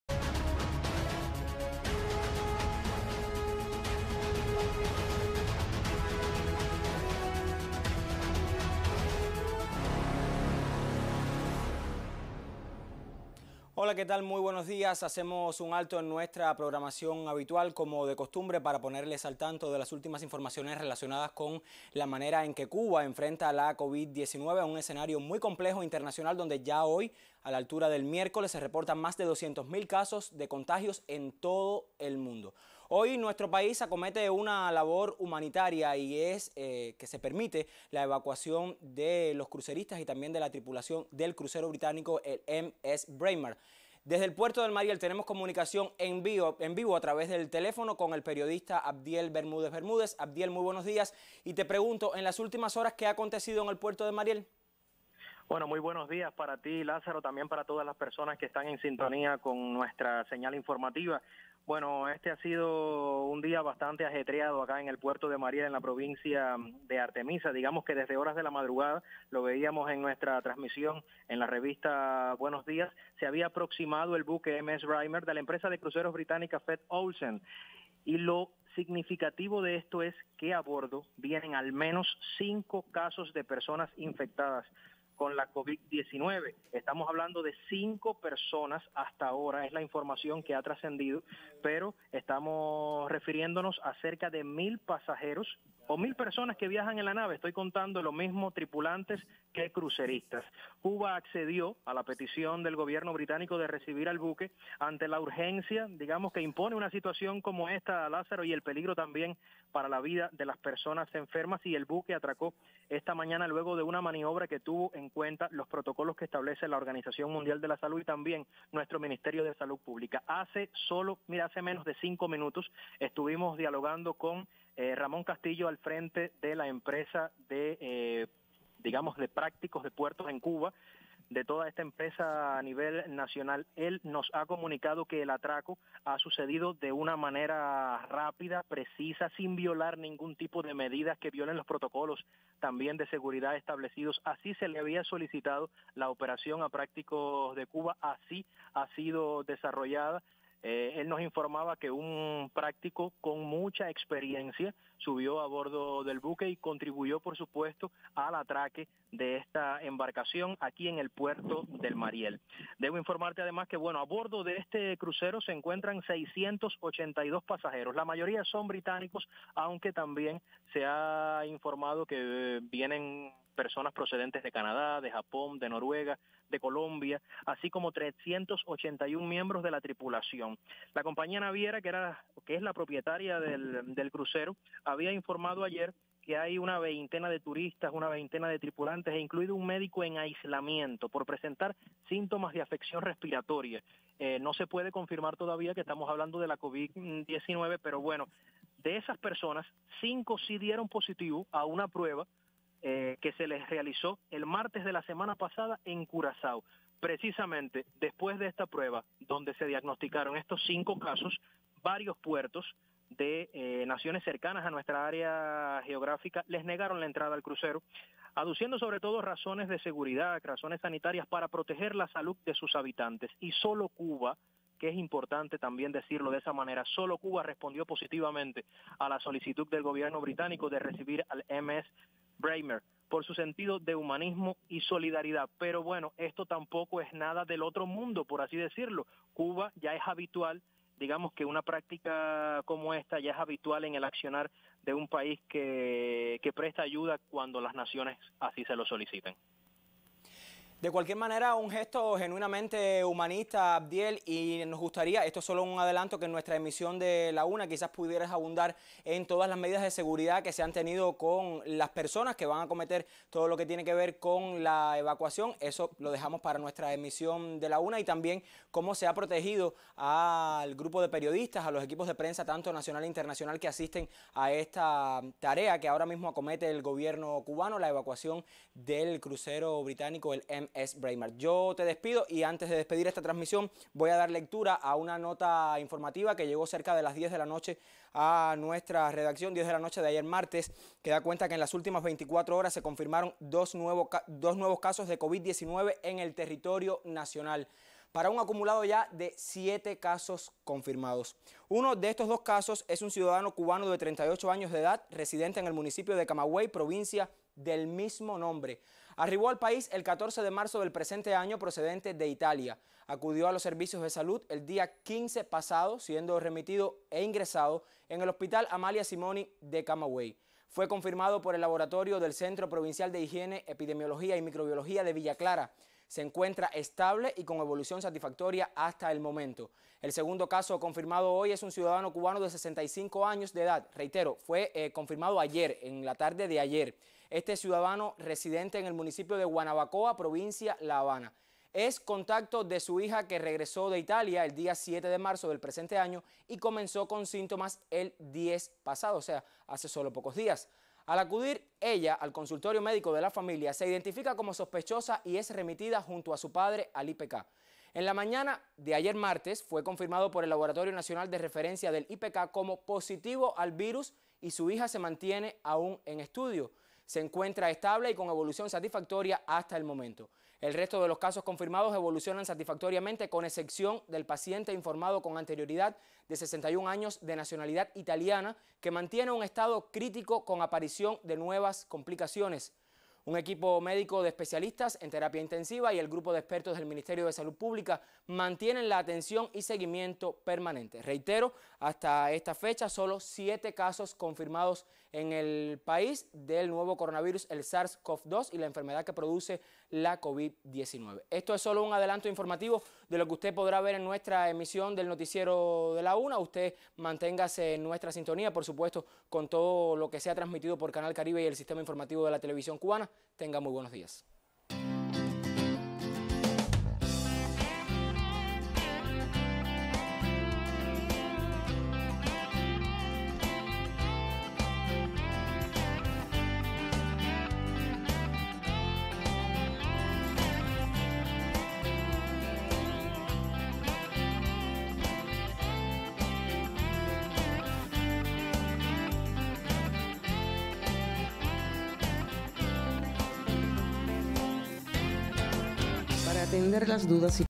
Atraca en puerto cubano de Mariel crucero británico MS Braemar
En la mañana de este 18 de marzo de 2020 atracó en puerto cubano de Mariel crucero británico MSBraemar con pacientes enfermos de COVID19.
El enviado especial para la TVC, el periodista Abdiel Bermúdez Bermúdez, tiene los promenores.
El Ministerio de Salud Pública de la República de Cuba anunció que se toman las medidas sanitarias para evitar contagio y ratifica el compromiso de Cuba con ofrecer ayuda solidaria ante una emergencia sanitaria donde peligre la vida de seres humanos.